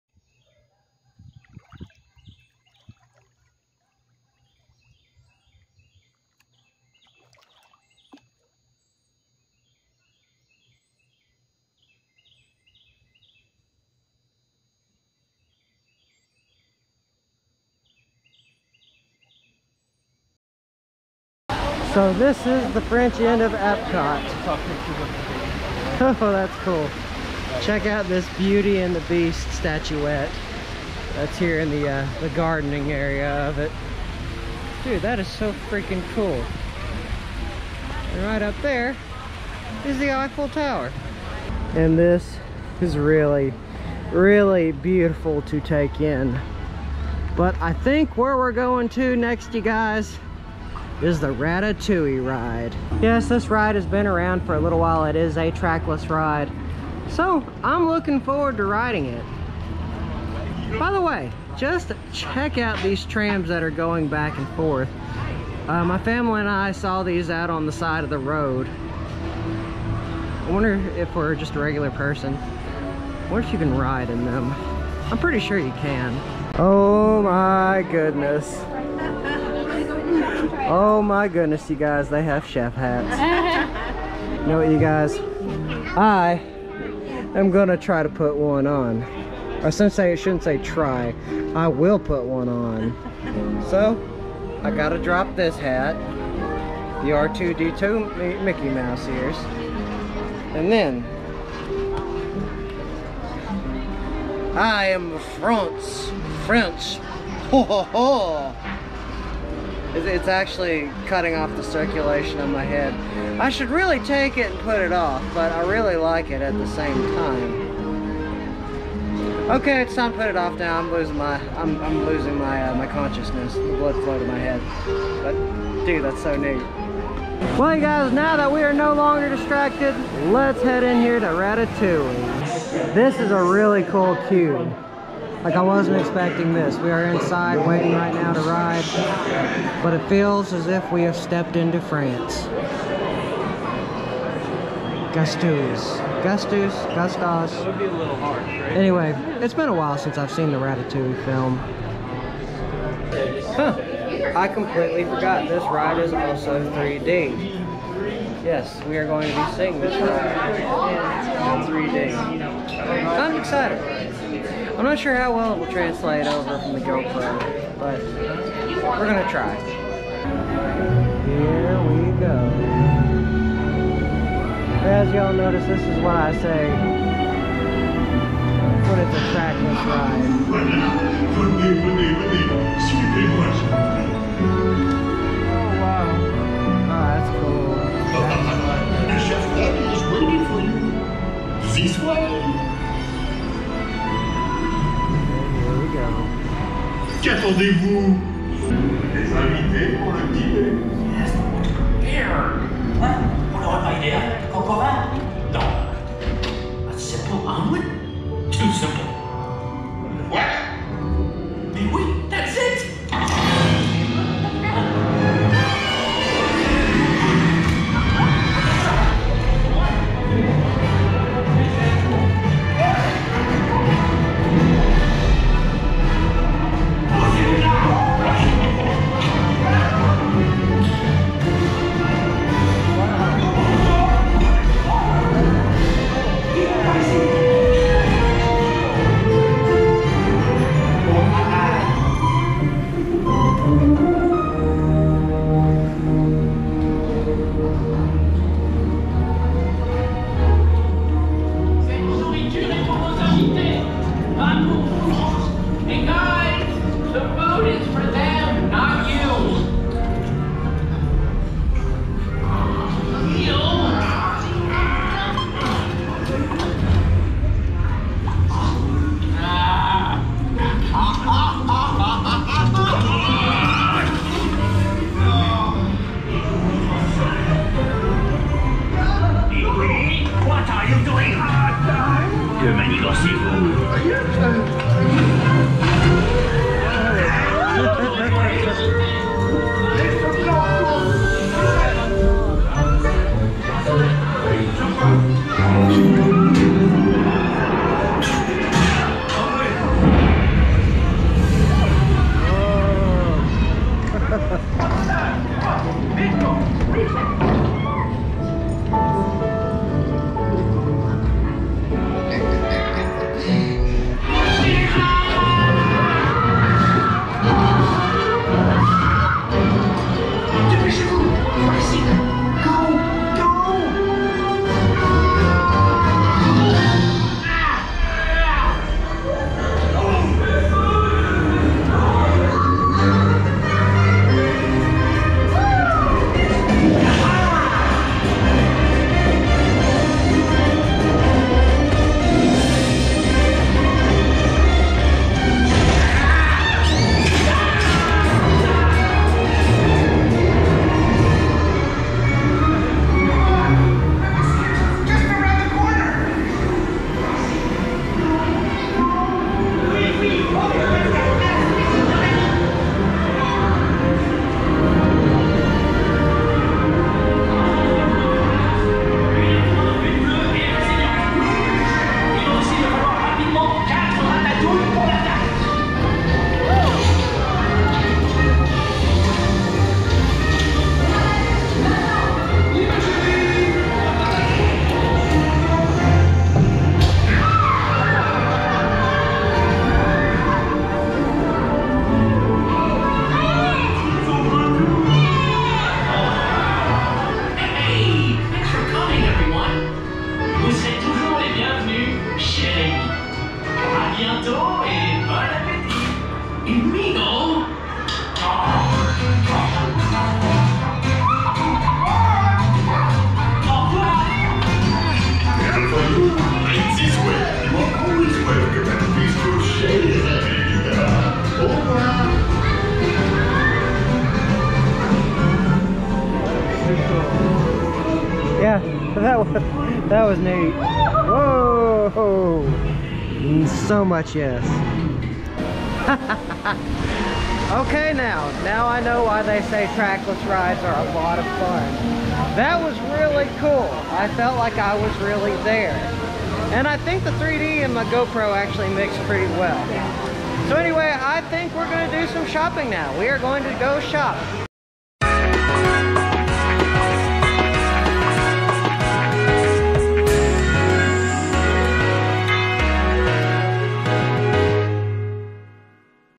Ratatouille Ride in 3D - Epcot POV Ride though
Hey you guys, Chet the Dream Poet here. Come join me for this little snippet from the previous vlog. This new trackless ride at Epcot is amazing!